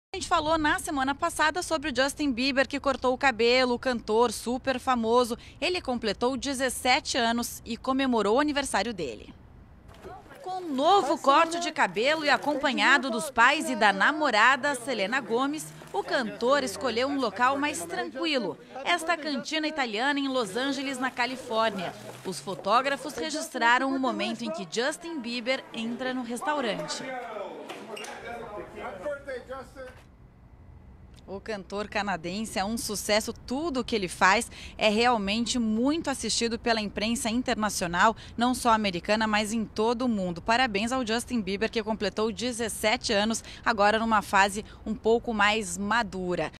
Justin Bieber completa 17 anos e comemora com os pais e a namorada. [ Rede Record ]
O ator canadense virou notícia na última semana, ao mudar o corte de cabelo, tão copiado pelos adolescentes. Ele comemorou o aniversário em uma cantina italiana em Los Angeles, na Califórnia.